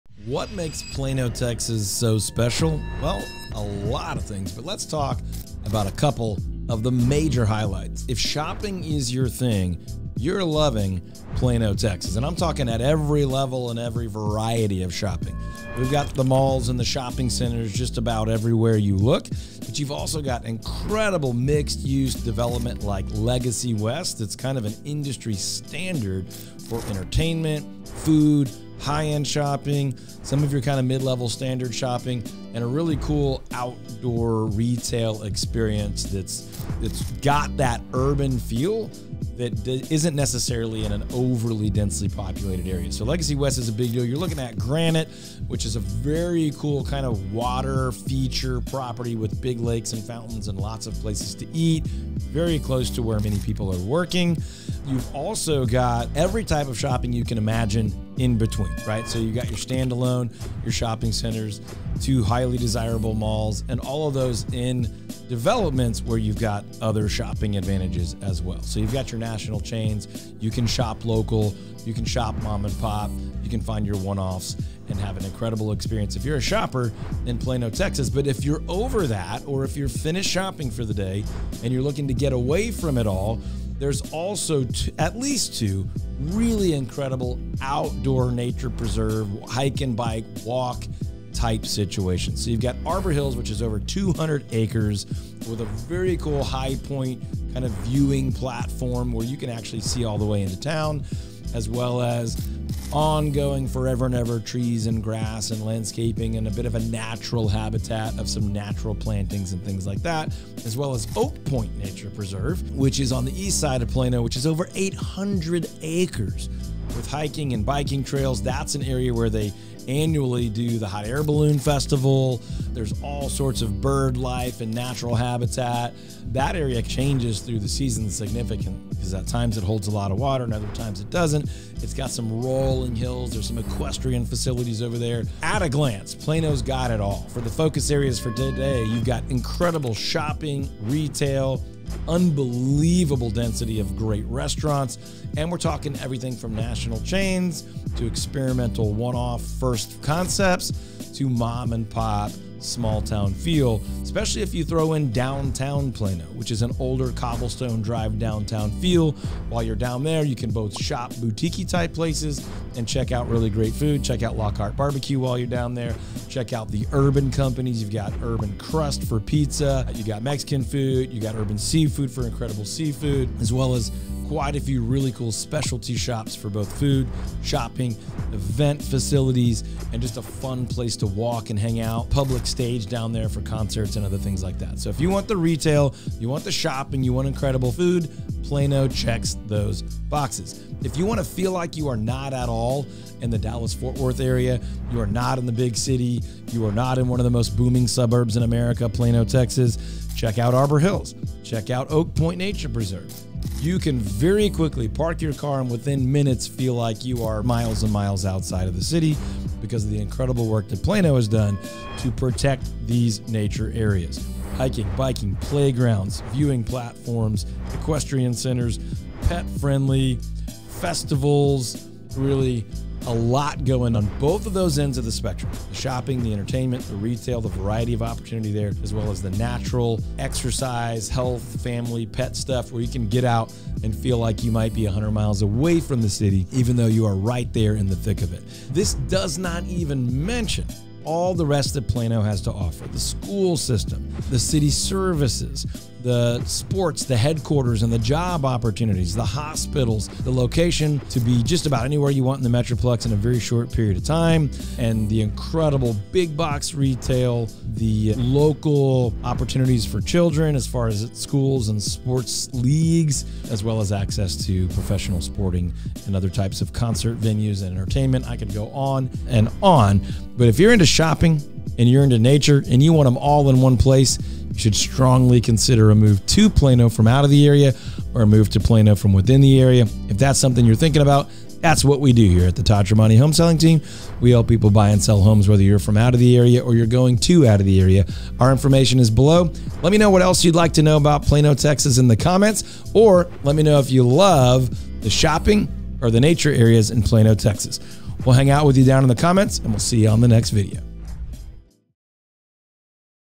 What is So Special About Plano, Texas?!
We're breaking down just a few reasons that make PLANO, TEXAS such a special place to visit and WHY you should live there!

Follow our LIVING ON LAND / BIG BACKYARDS channel!